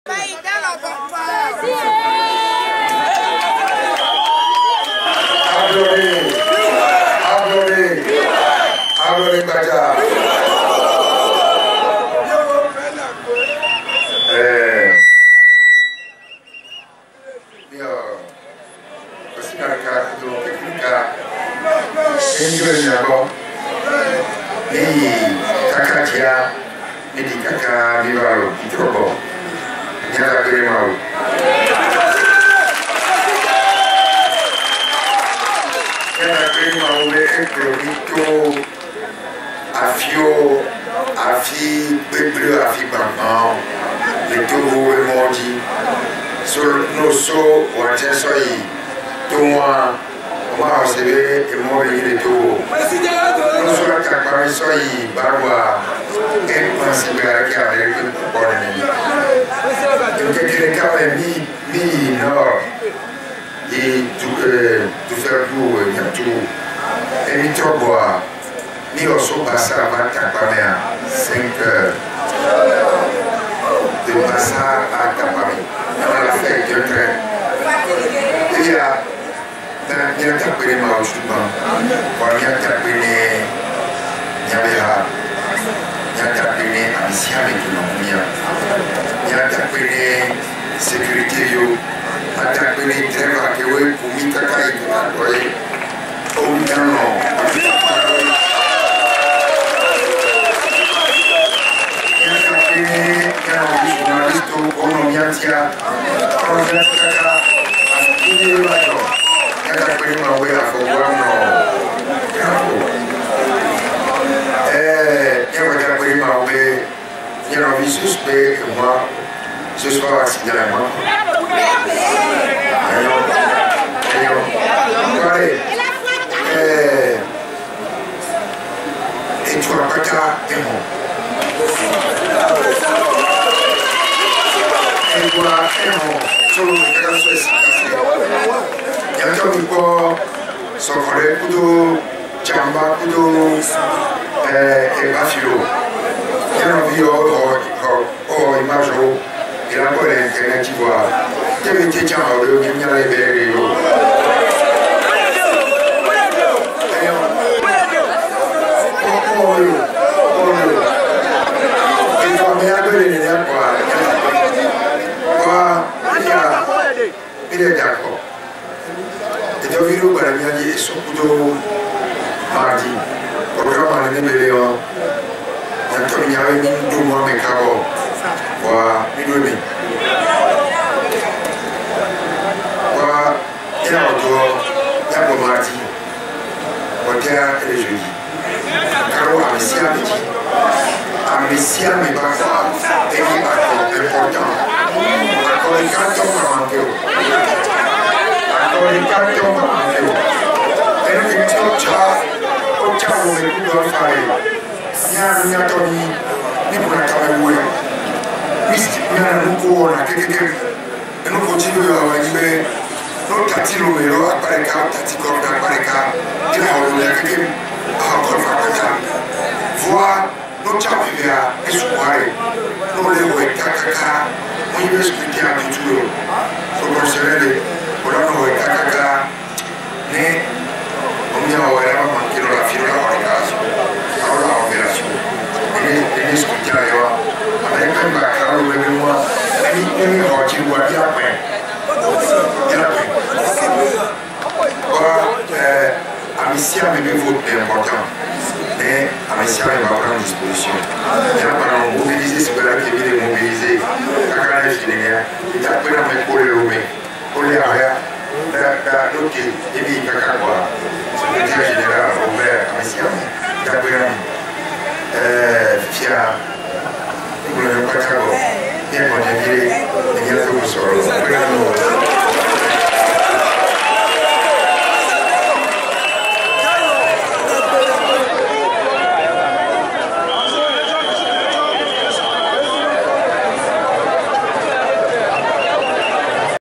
Echo du CST: Jean-Pierre FABRE fait le point de sa tournée à la foule qui l'a accueilli [21/7/2013]
Echo du CST: Jean-Pierre FABRE fait le point de sa tournée aux militants venus l'accueillir[21/7/2013]